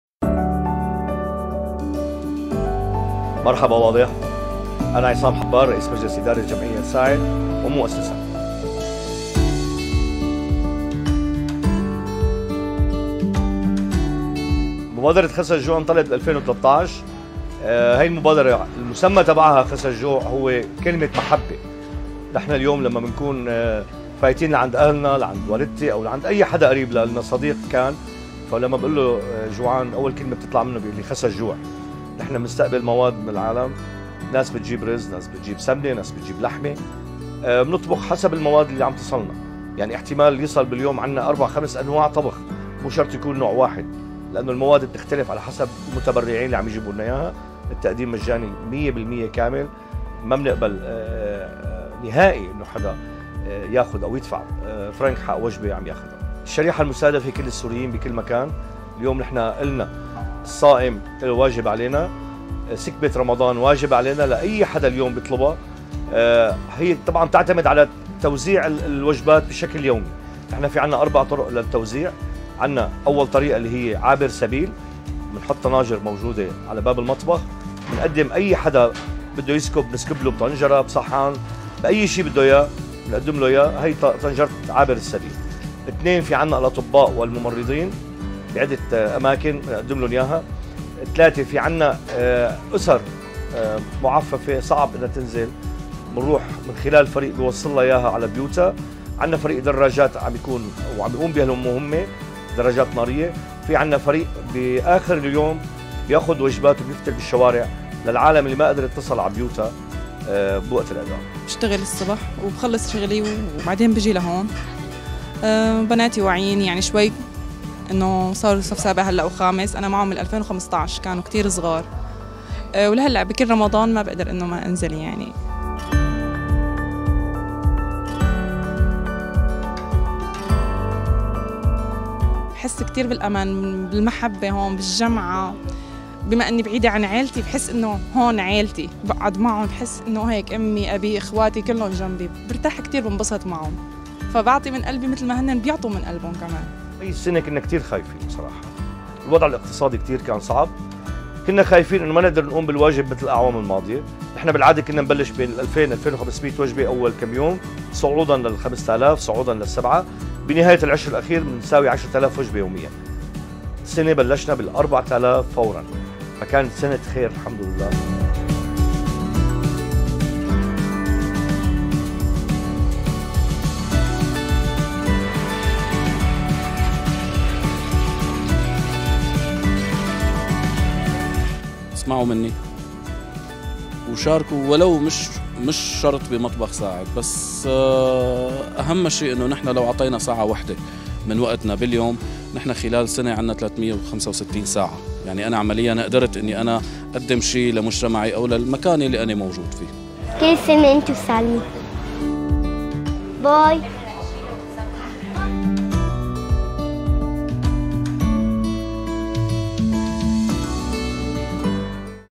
Ils offrent 5000 repas par jour ! Découvrez leur histoire !🥙🌙
Pour la neuvième année consécutive, et malgré la situation économique difficile, "Khasa El Jou3" continue à semer la joie et l'espoir dans le coeur des jeûneurs...😍🥙🌙Chapeau bas à son fondateur! 👏